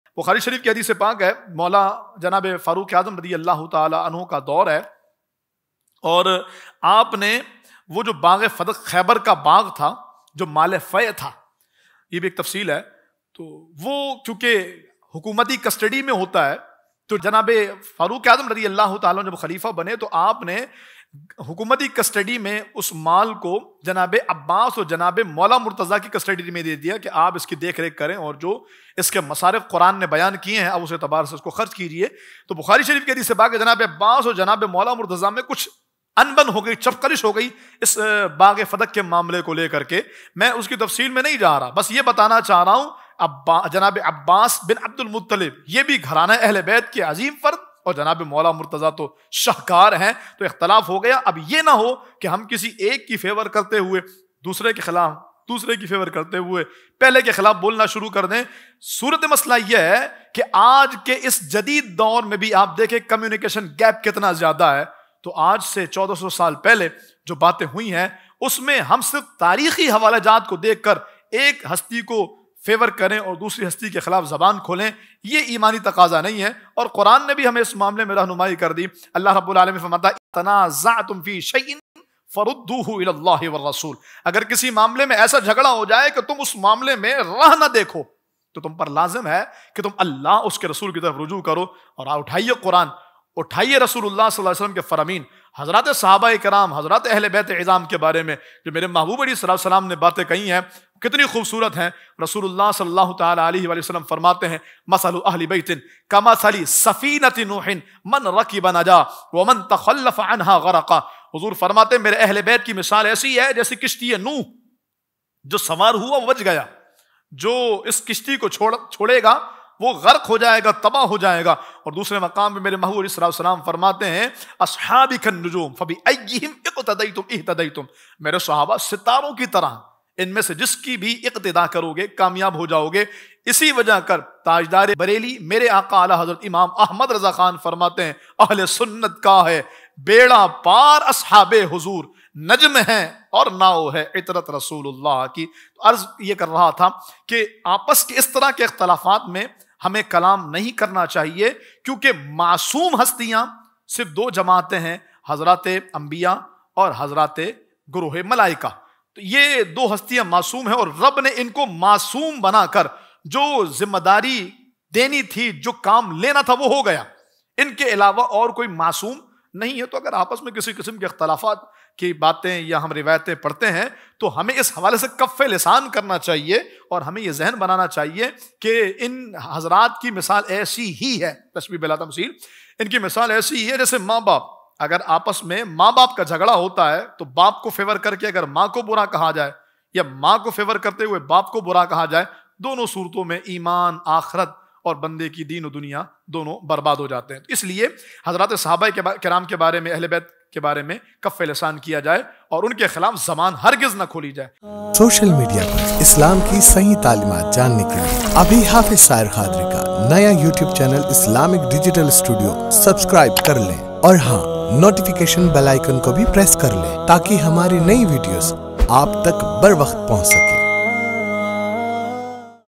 Bagh e Fidak Ki Haqeeqat | Kya Sunni Haq Par Hain | Allama Syed Ghazanfar Tahir Madani | IDS
100% Authentic Islamic Content